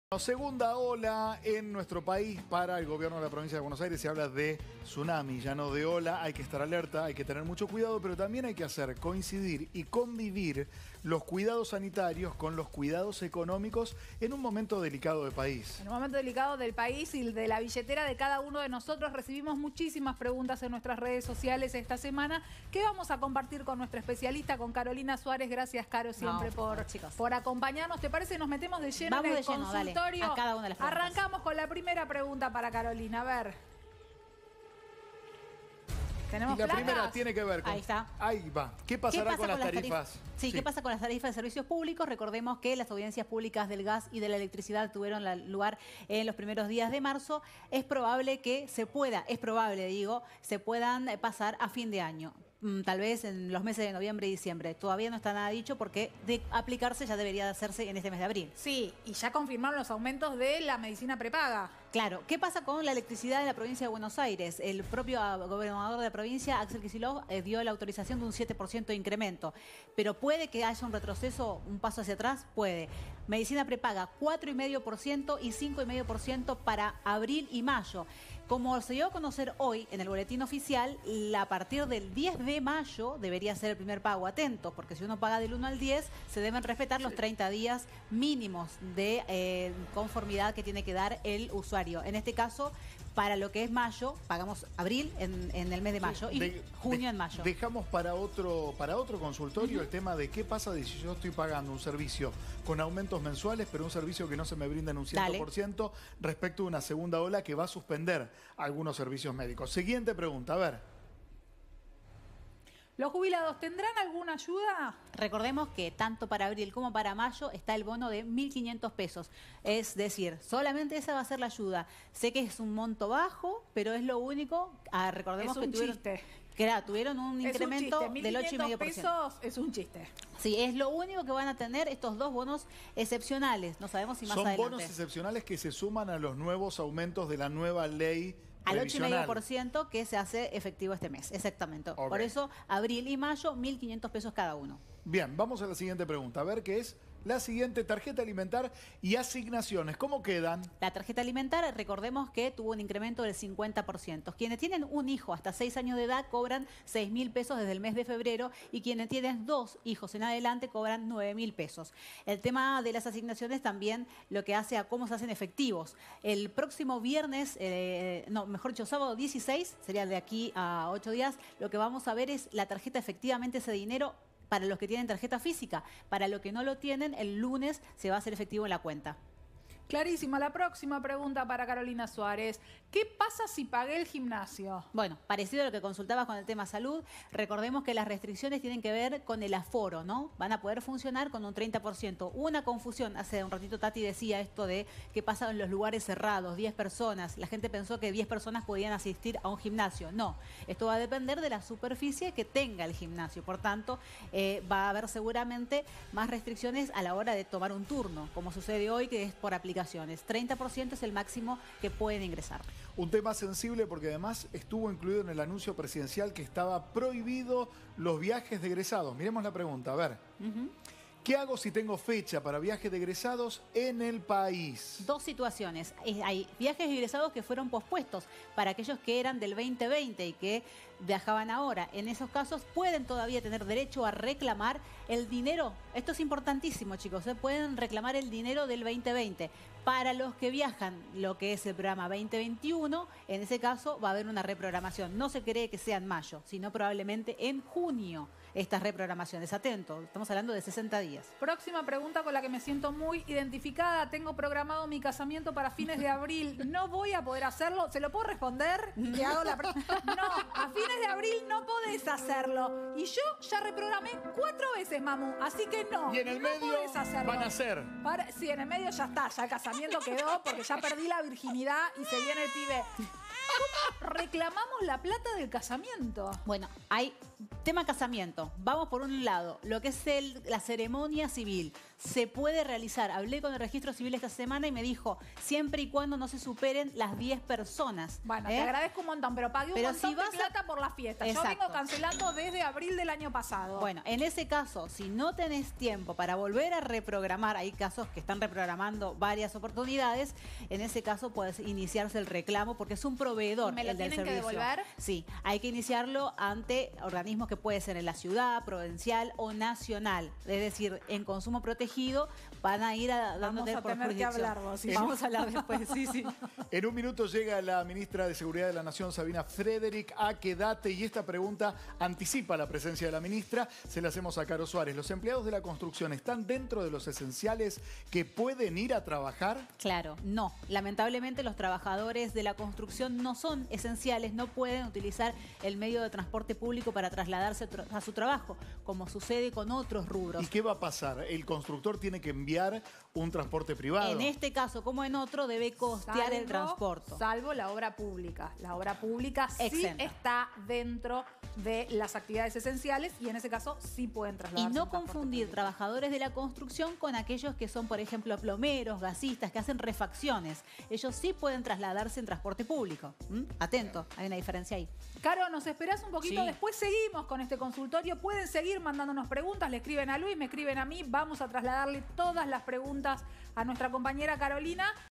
Quedate: Ayuda y subsidios ante la crisis y pandemia
Carolina Suárez, Especialista en Consumo, respondió algunas inquietudes de los argentinos sobre las ayudas y subsidios a las cuales pueden acceder para sortear el impacto económico que genera la pandemia.